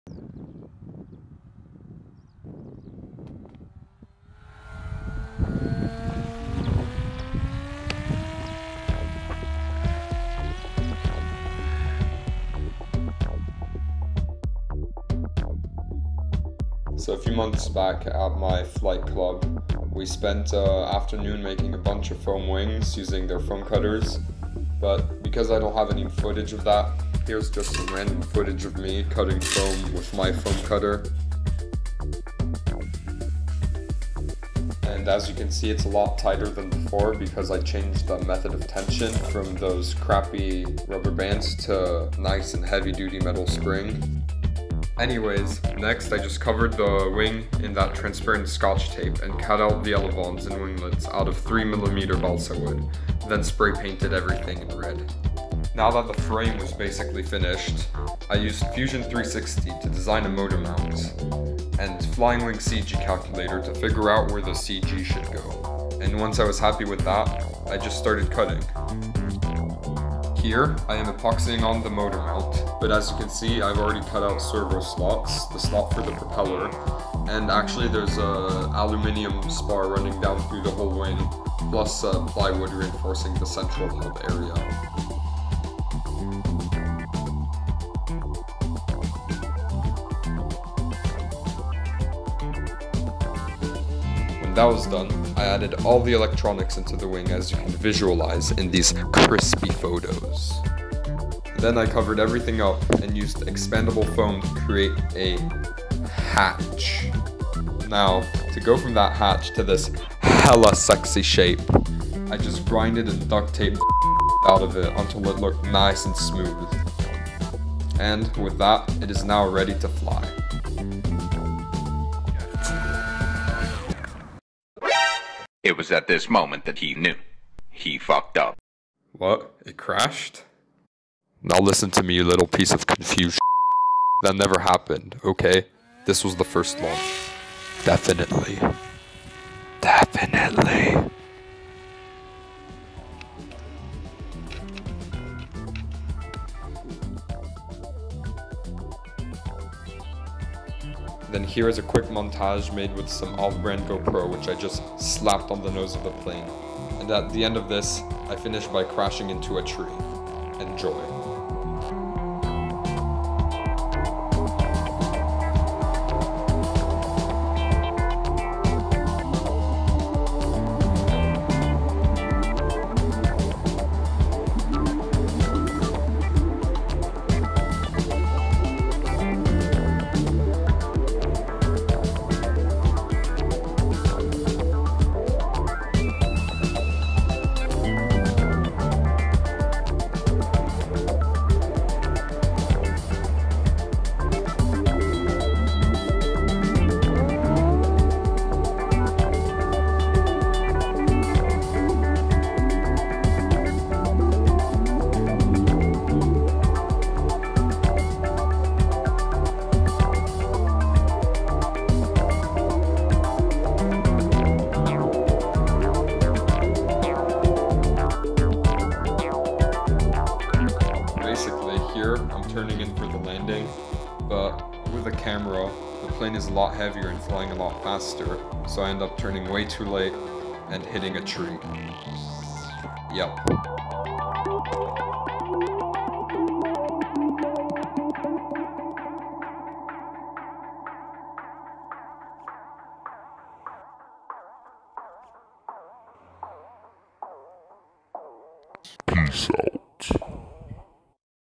RC Flying Wing Build out of Foam
To not immediately crash the balsa wood wing I am building, I decided to make a foam delta wing of similar size and weight to get a good hang of flying. In the end, this wing weighs a total of slightly less than 800 grams (without any camera on it) and has a wingspan of around 1.4 meters. I run it on a 3 cell with an 18 amp motor and 7"4 prop.  I end up crashing into a tree with the camera on the plane on landing, but there was very minimal damage and repaired it in a day.